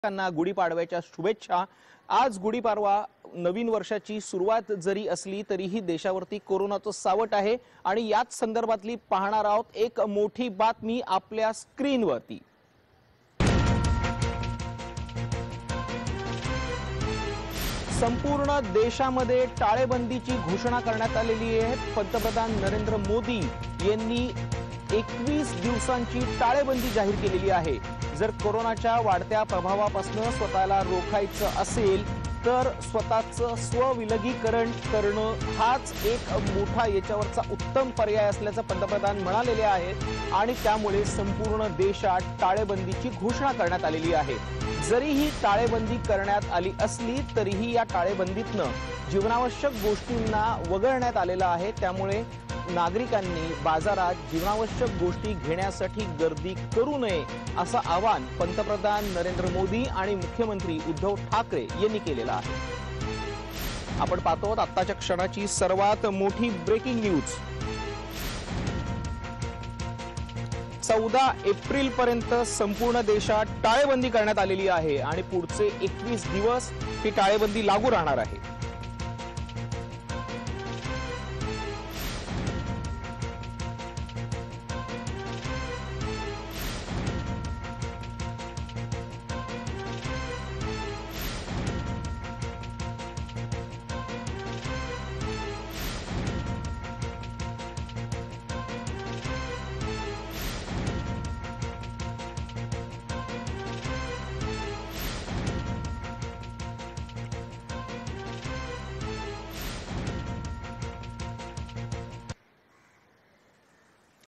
Coronavirus | 21 दिवसांसाठी देशात टाळेबंदी | Marathi News
Coronavirus in India | Modi Orders Total Lockdown of 21 Days